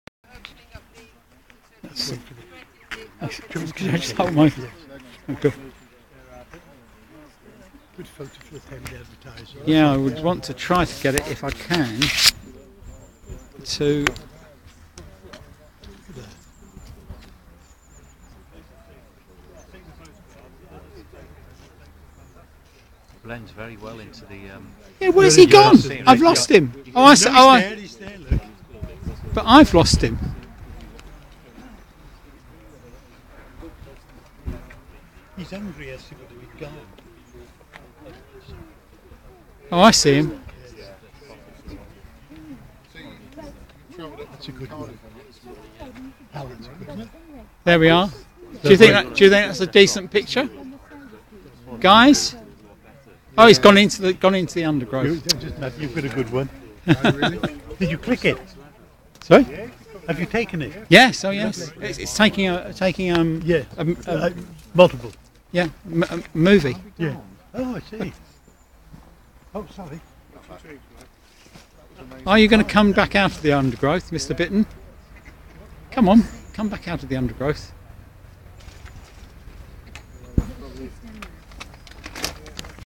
Bittern 17122010029.mp4
short movie of the bittern taken during the site visit on Friday - we all saw him :)